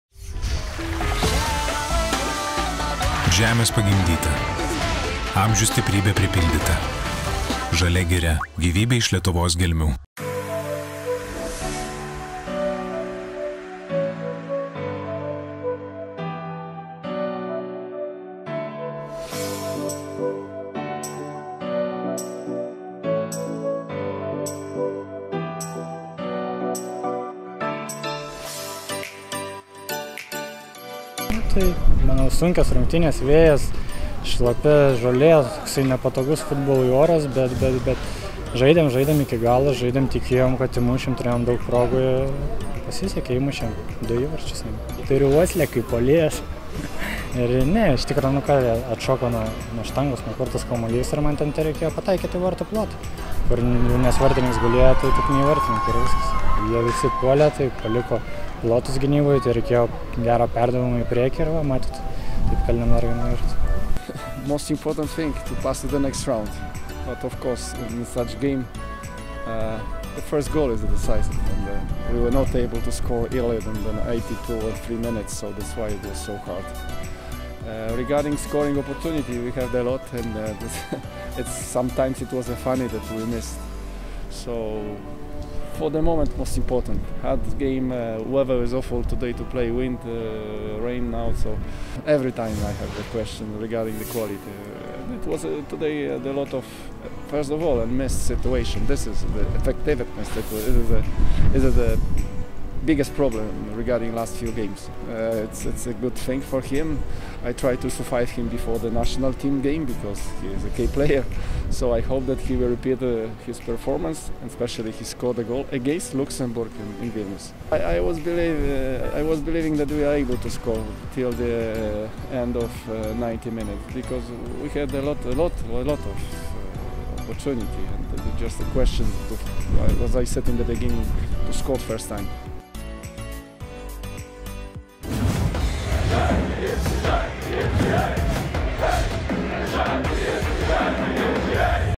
R.Baravykas: „Matyt turiu įvarčio uoslę“
Rolandas Baravykas po pergalės LFF taurės 1/16 prieš „Atlantą“ pasidalino savo mintimis apie rungtynes.